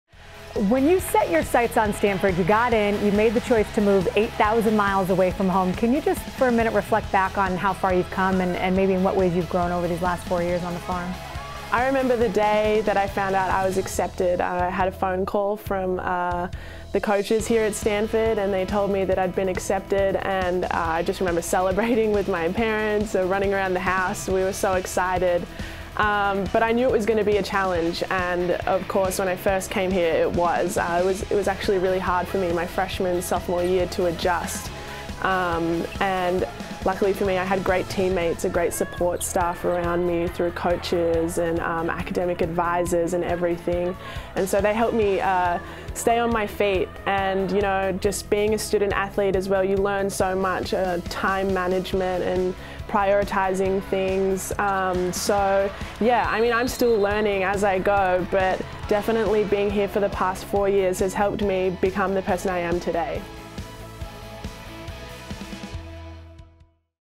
Alanna Smith grateful for Stanford community's support during time on The Farm: 'They helped me...
After winning Pac-12 Women's Basketball Scholar-Athlete of the Year honors on Monday, Alanna Smith talked with Pac-12 Networks' Ashley Adamson to hear her thought on the prestigious award. See more from Alanna on Tuesday at 5 p.m. PT/ 6 p.m. MT on Pac-12 Network during the "Inside Pac-12 Basketball: Women's All-Conference Awards Show."